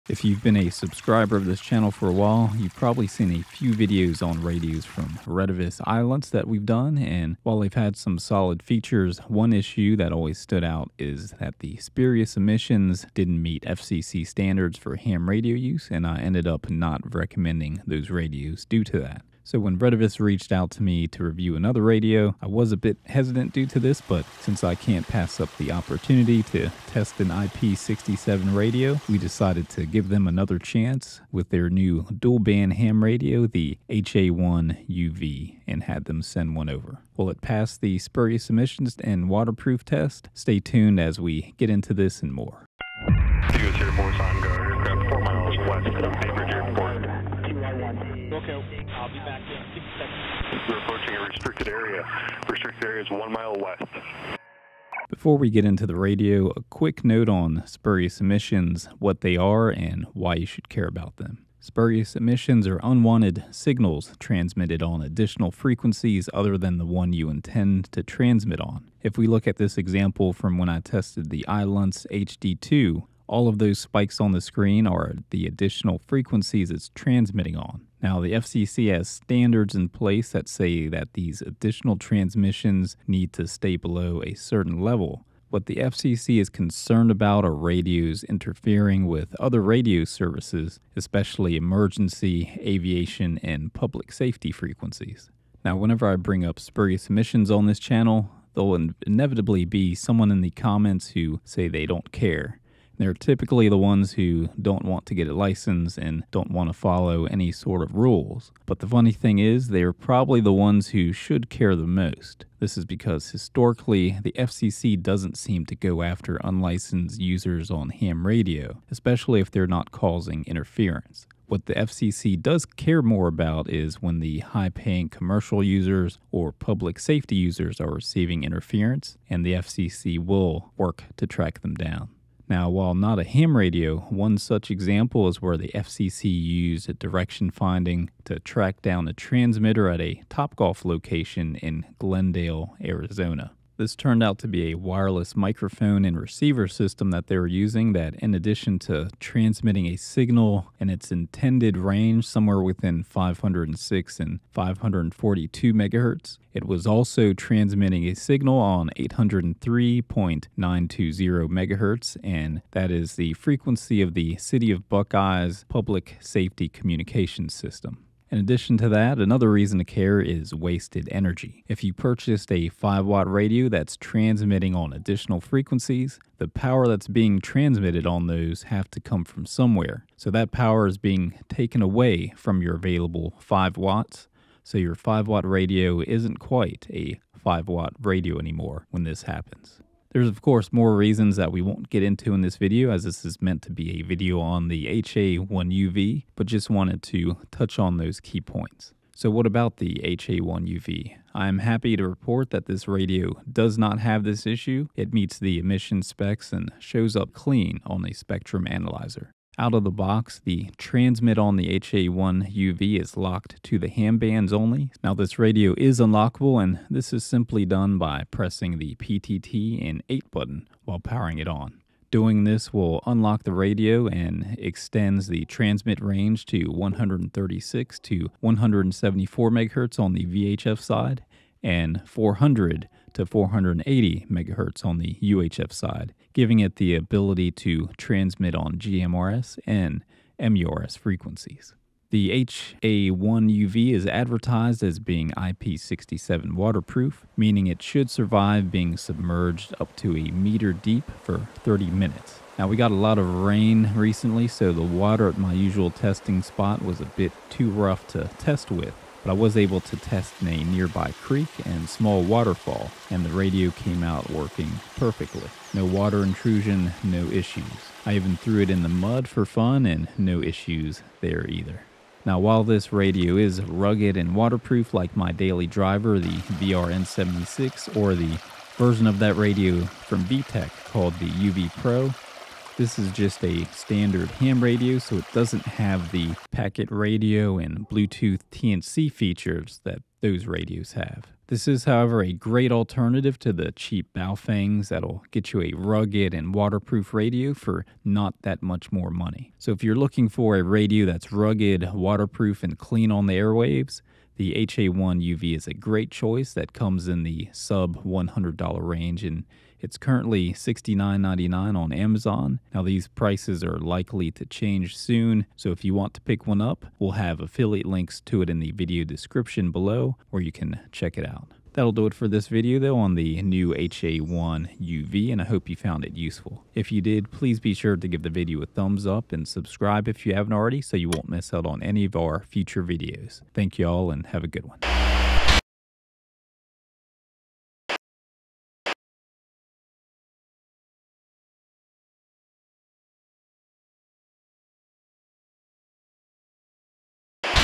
The new HA1UV dual band ham radio front Retevis Ailunce
For today's videos we're checking out the new HA1UV dual band ham radio from Retevis Ailunce and checking out if they have a spectrally clean radio with this one and go over why you should care.

Check out our link below if you'd like to check it out and pick one up for yourself!